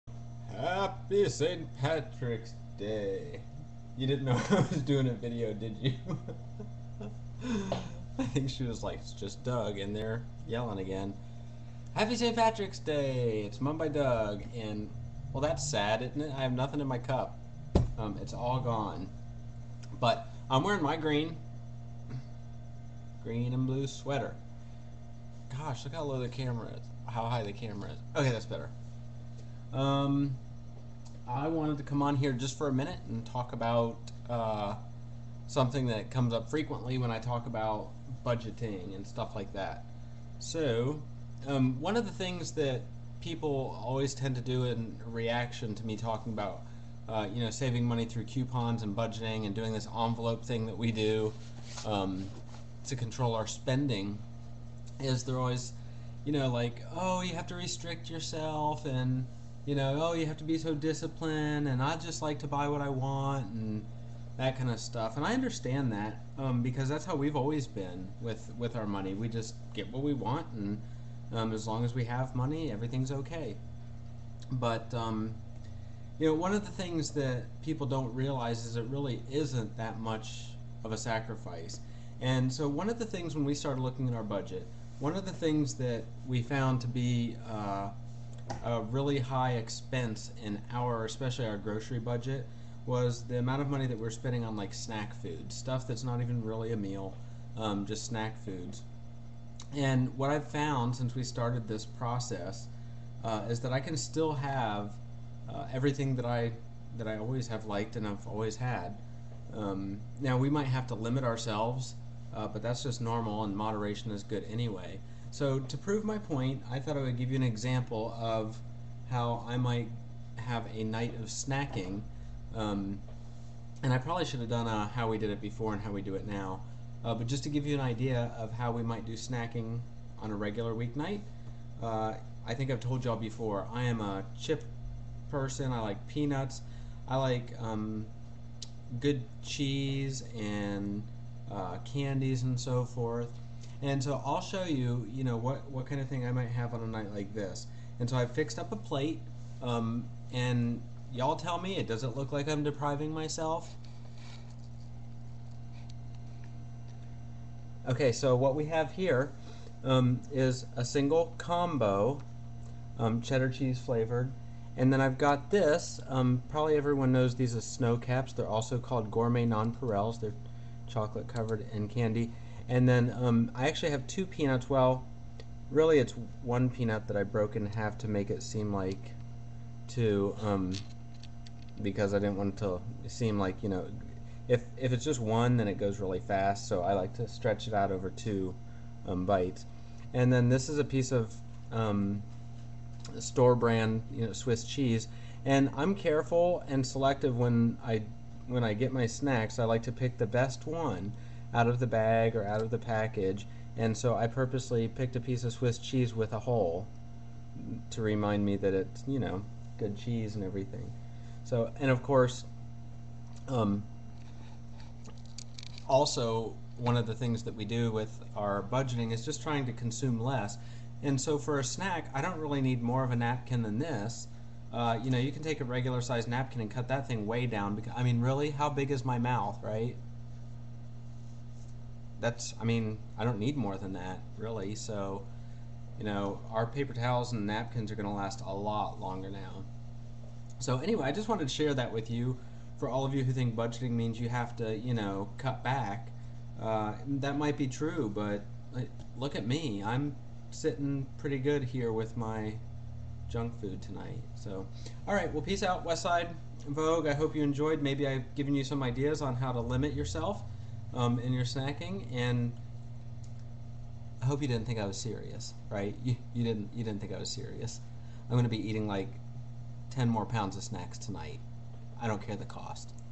Please watch..it could save you thousands of $$
The importance of moderation to your budget...um...yeah. And when I posted this video last night, I apparently made it "private" by accident. so it's a day late.